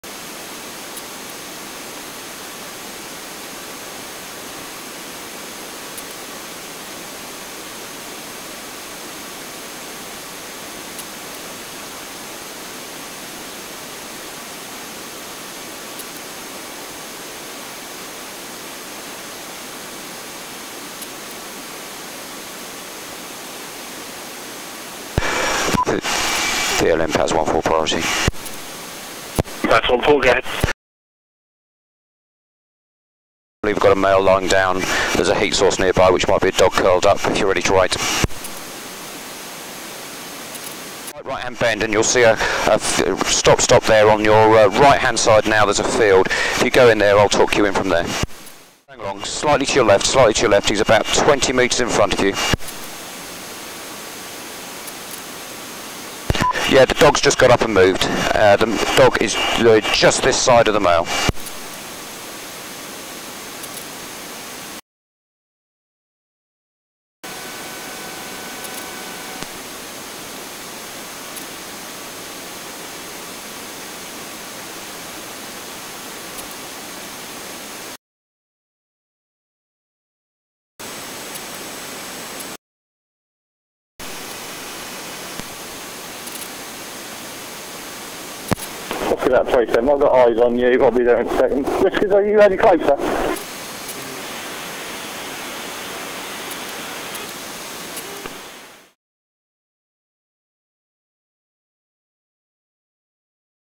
Police Helicopter finds Missing Person
The Suffolk, UK based Police Helicopter part of the National Police Air Service locates a missing person in the freezing cold and the owners dog still remains by his side and directs officers to his owner Martin Kay. Who survived this ordeal.